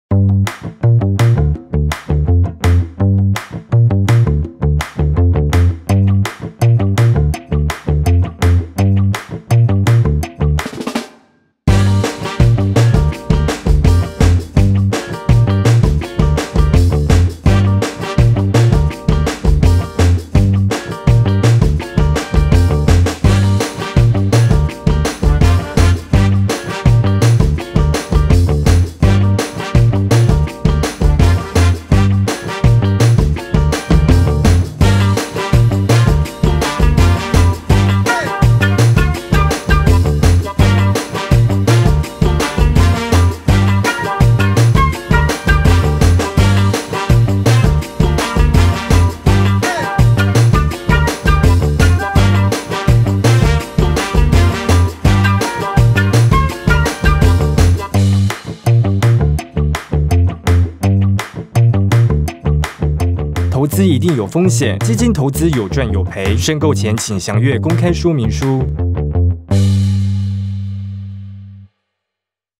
買基金用icash Pay完美絕配｜全台首家投信結合電子支付帳戶買基金｜元大投信 Yuanta Funds
元大投信 x icash Pay 全台首家投信結合電子支付帳戶買基金

■ 投資人僅得透過元大投信之基金交易平台進行「電子支付帳戶申購基金」，目前尚未開放「買回」。icash Pay 僅提供代理收付款項服務，並未涉及基金商品或服務之經營。